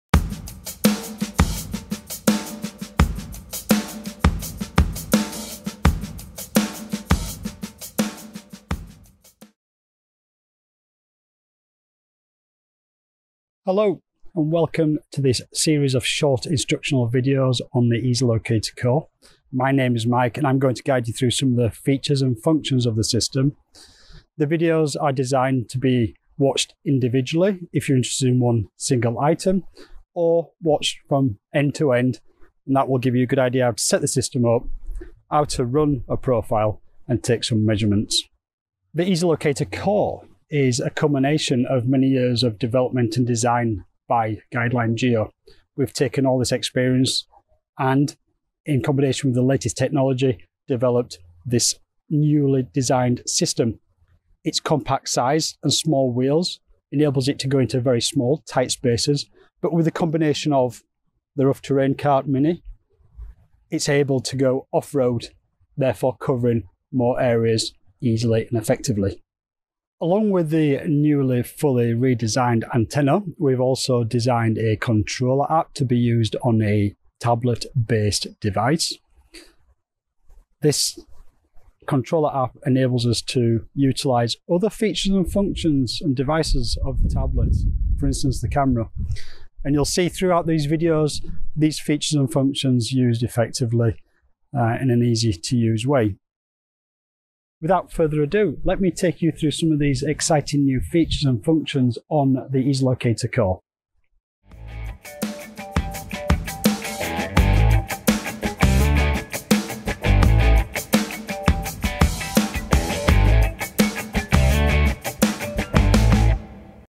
MALÅ Easy Locator Core: Introduction
Welcome to the MALÅ Easy Locator Core instructional video series, where you can take a complete journey through the features and functions on the system or pick a single item of interest. Aimed as a quick guide to get you started in the world of the MALÅ Easy Locator Core. We welcome any feedback or questions, thank you for watching and please remember to hit that like button and subscribe to keep up to date with future updates and newly added features.